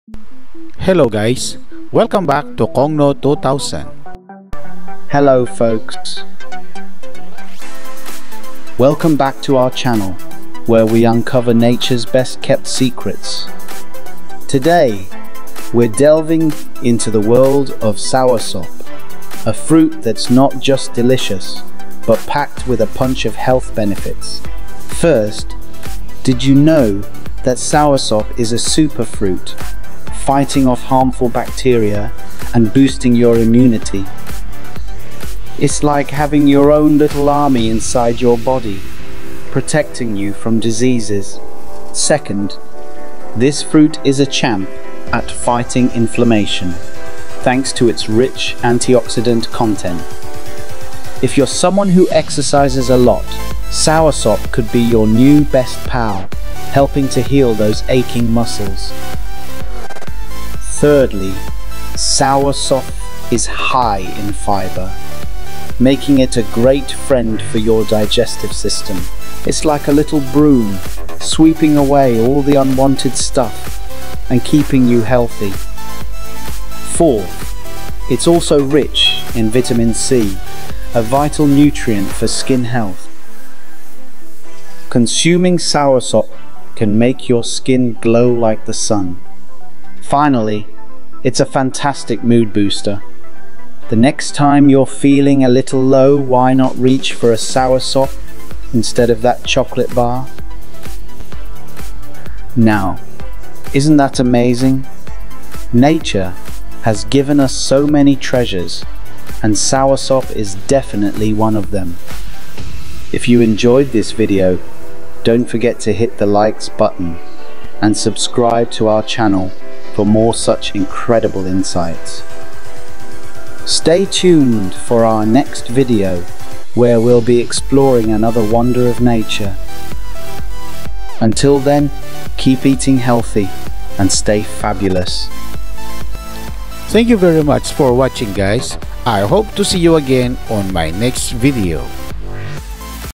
TOP 5 HEALTH BENEFITS OF EATING SOURSOP
this video is about the health benefits of eating fruits edited manually using filmora and automatically using videozen.ai. and veed.io